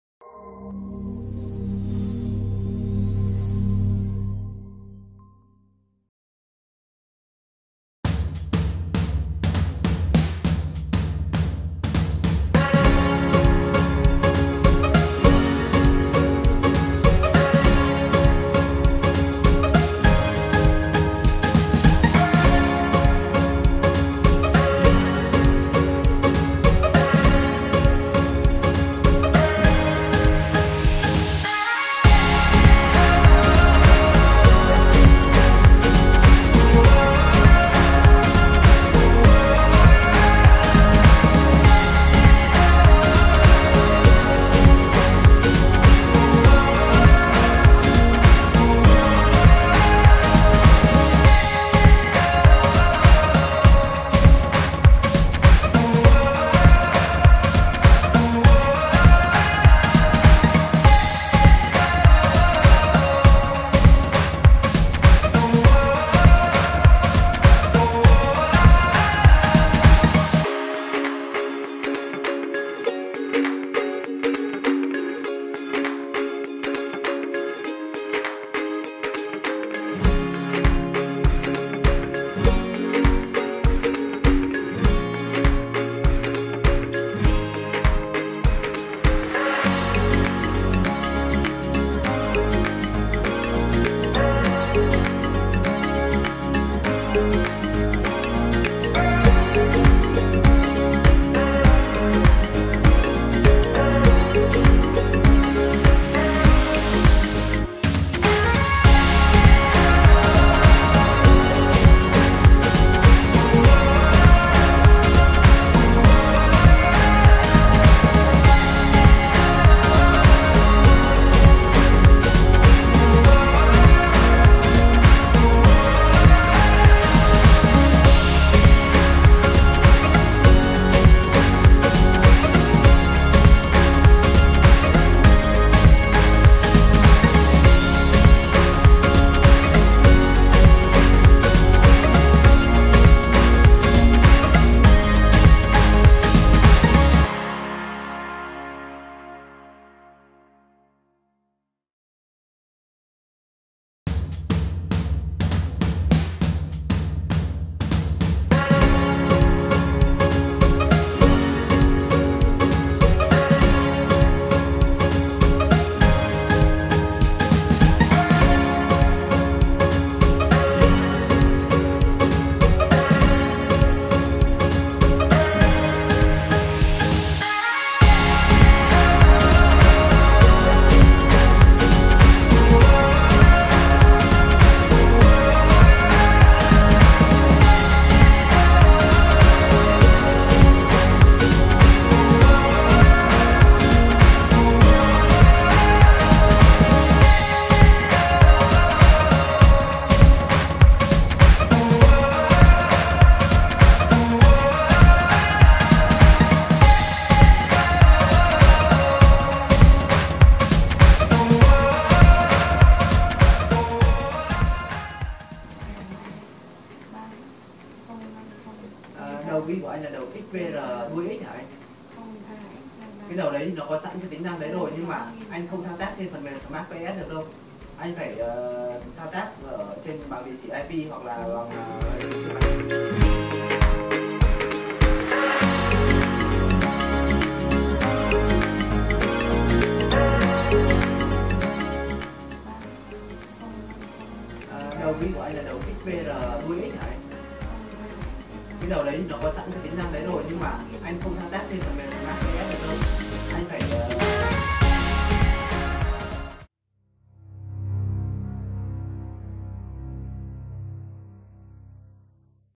HDSD_051 Video Demo, Hướng Dẫn Cách Sử Dụng "DIP SWITCH" Trên Dòng Cam HDCVI 2501 - 2241 DSS_Dahua
HDSD_051 Video Demo, Hướng Dẫn Cách Sử Dụng "DIP SWITCH" Trên Dòng Cam HDCVI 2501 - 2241 Của DSS_Dahua